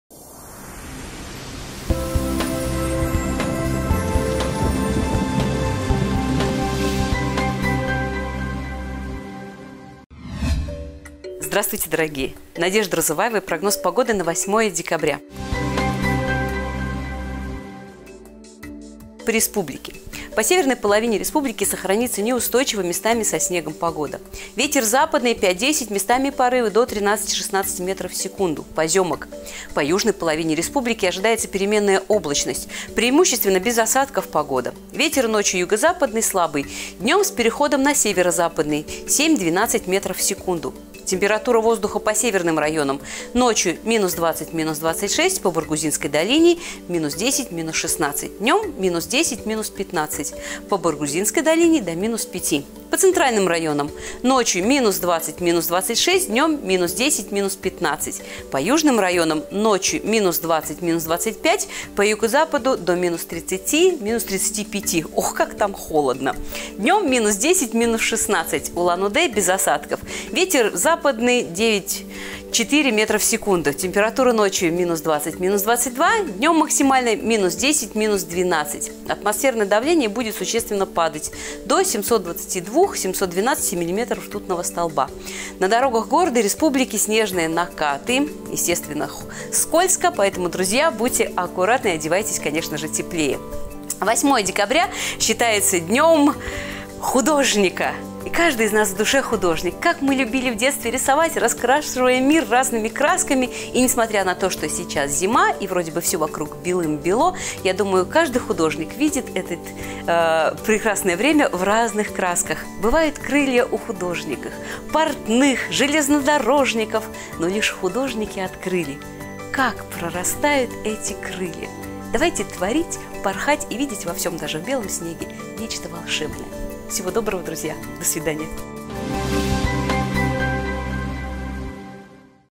Прогноз погоды на 08.12.2020
По республике: по северной половине республики сохранится неустойчивая, местами со снегом погода. Ветер западный 5-10, местами до 13-16 м/с, поземок.
По южной половине республики ожидается переменная облачность, преимущественно без осадков погода. Ветер ночью юго-западный слабый, днем с переходом на северо-западный 7-12 м/с.